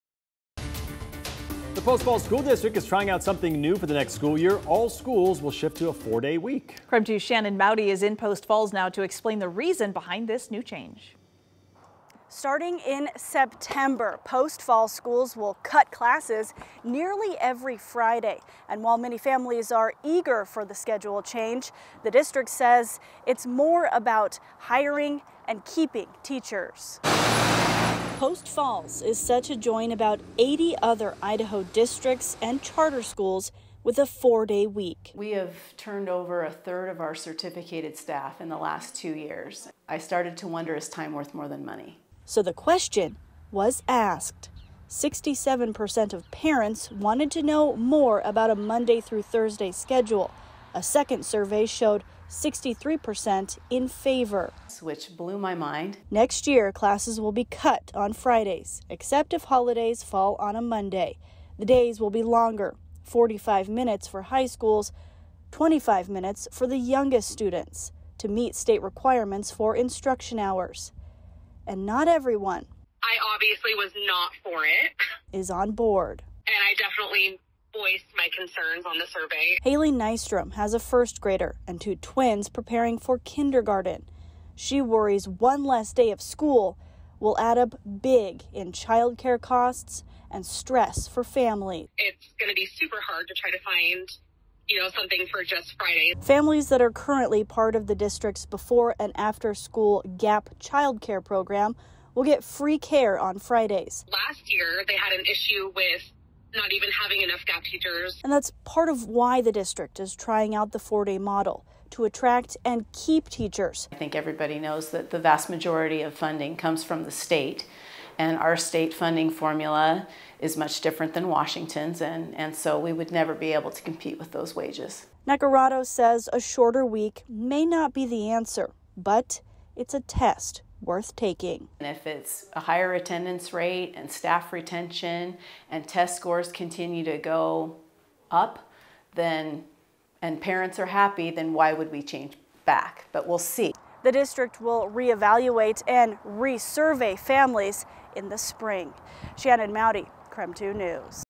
Post Falls School District experimenting with 4-day school week
Parents in the area are sharing their thoughts on the experiment and how it will affect their schedules.